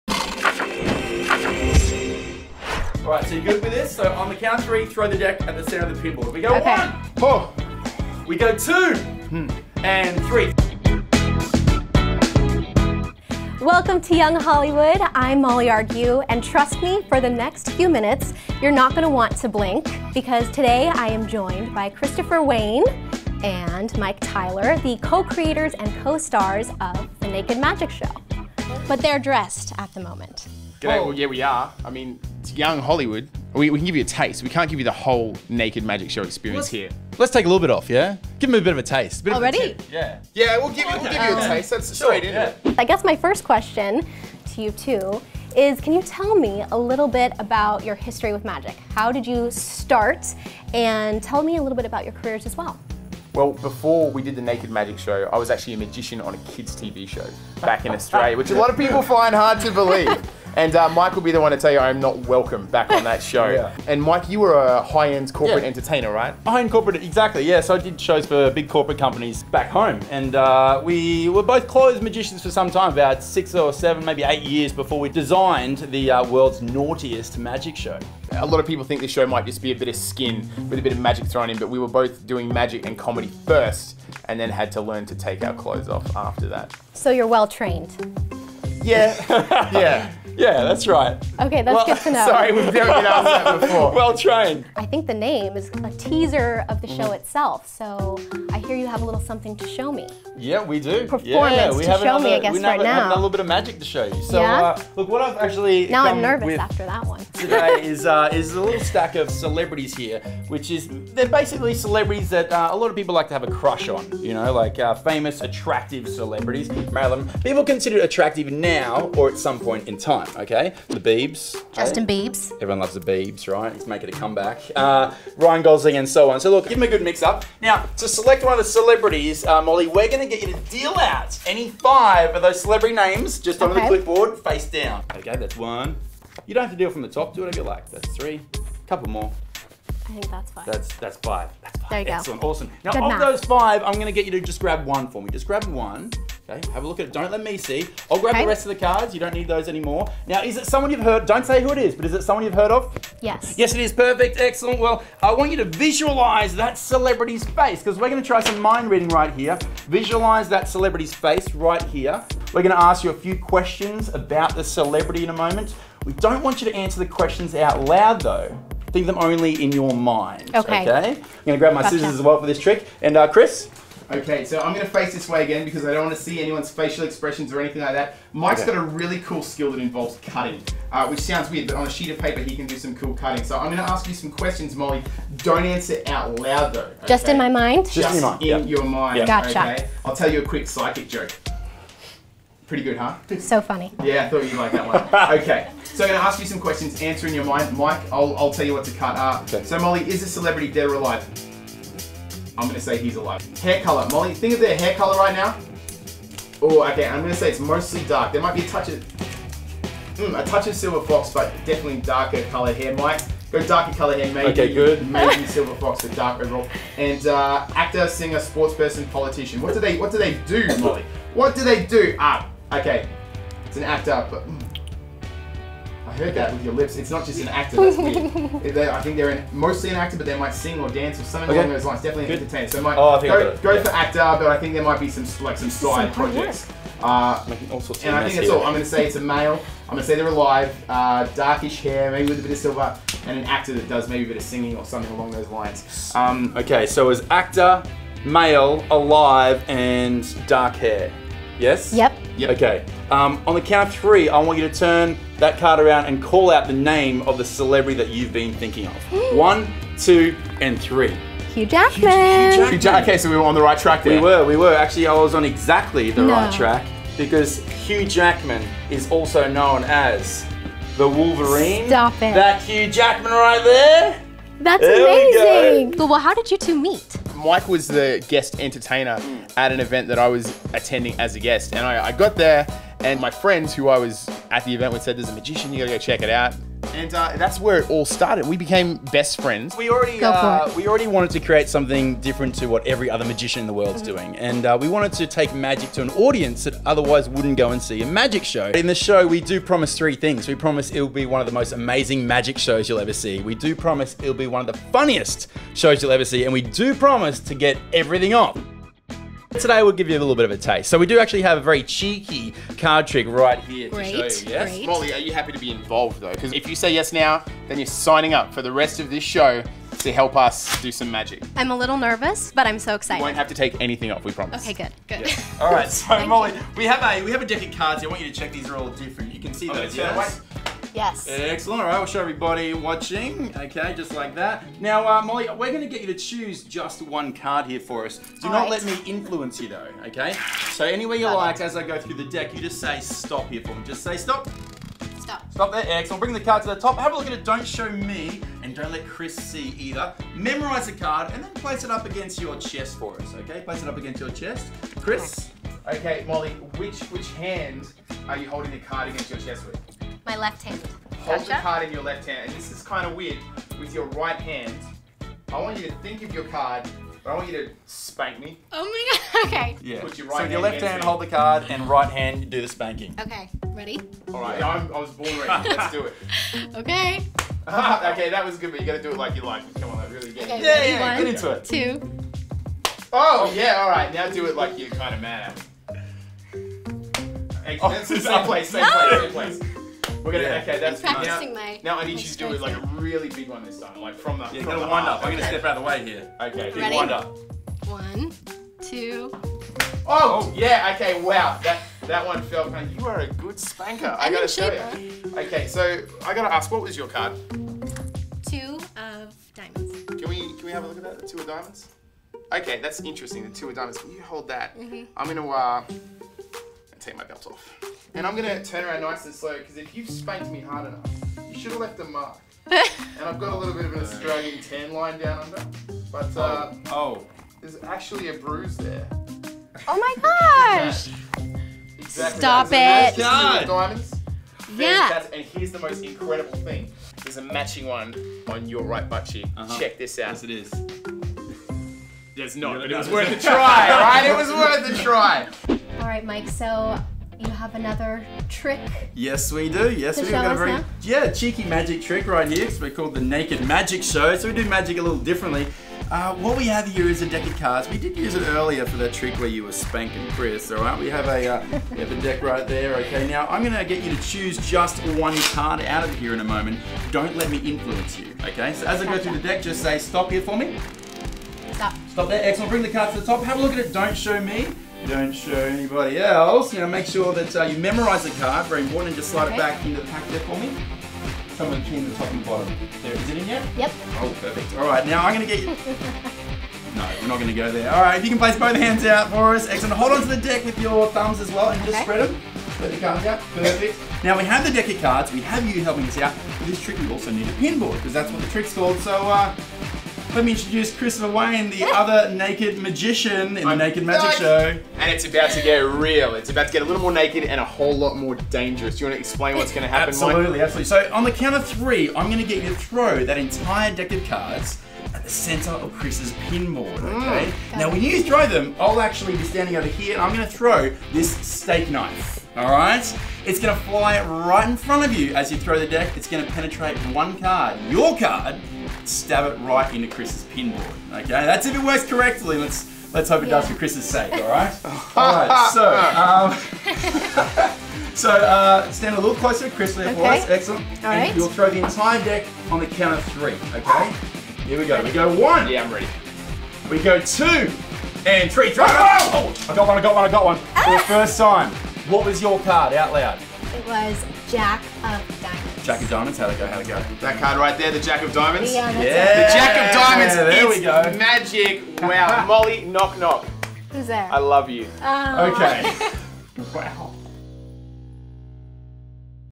Cheeky Card Tricks with The Naked Magic Show
Crank up the AC because the hunky Australians from The Naked Magic Show are in the YH Studio to wow us with their amazing... card tricks! Christopher Wayne and Mike Tyler show off their cheeky skills while telling us how they first met, how their act first came together, and how they came to be known as the world's naughtiest magic show!